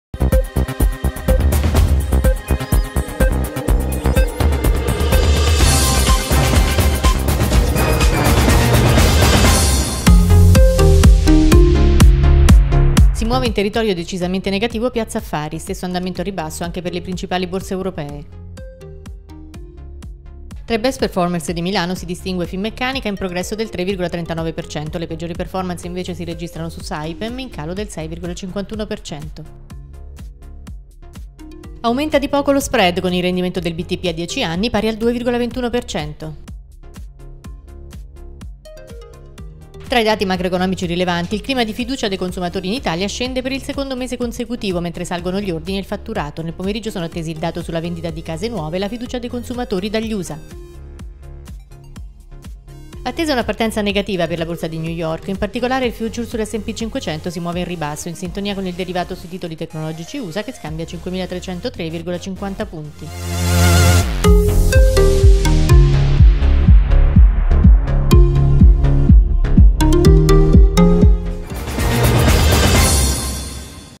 1 minuto in Borsa 24 febbraio 2017
Si muove in territorio decisamente negativo Piazza Affari. Stesso andamento al ribasso anche per le principali Borse europee.
Tra i best performers di Milano, si distingue Finmeccanica, in progresso del 3,39%. Le peggiori performance, invece, si registrano su Saipem, in calo del 6,51%.
Aumenta di poco lo spread, con il rendimento del BTP a 10 anni pari al 2,21%.
Tra i dati macroeconomici rilevanti, il clima di fiducia dei consumatori in Italia scende per il secondo mese consecutivo, mentre salgono gli ordini e il fatturato. Nel pomeriggio sono attesi il dato sulla vendita di case nuove e la fiducia dei consumatori dagli USA.
Attesa una partenza negativa per la Borsa di New York. In particolare, il future sull'S&P-500 si muove in ribasso, in sintonia con il derivato sui titoli tecnologici USA che scambia a 5.303,50 punti.